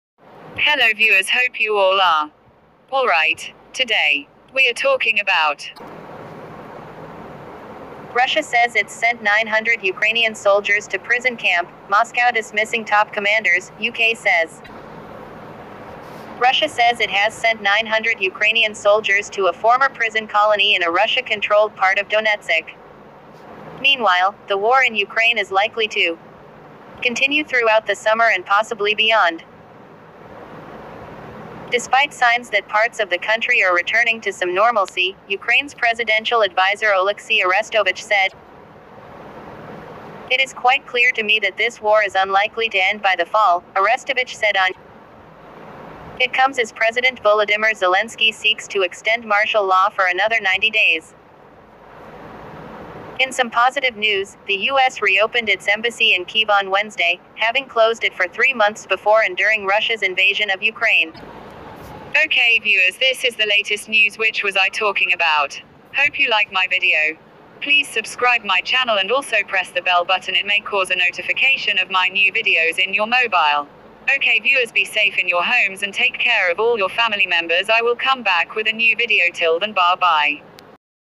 Russia says it's sent 900 Ukrainian soldiers to prison camp Moscow dismissing top commanders UK says
Russia says it has sent 900 Ukrainian soldiers to a former prison colony in a Russia-controlled part of Donetsk.

Meanwhile, the war in Ukraine is likely to continue throughout the summer and possibly beyond, despite signs that parts of the country are returning to some normalcy, Ukraine's presidential advisor Oleksii Arestovych said, according to  News.

"It is quite clear to me that this war is unlikely to end by the fall," Arestovich said on Ukrainian TV,  News reported. It comes as President Volodymyr Zelenskyy seeks to extend martial law for another 90 days.

In some positive news, the U.S. reopened its embassy in Kyiv on Wednesday, having closed it for three months before and during Russia's invasion of Ukraine.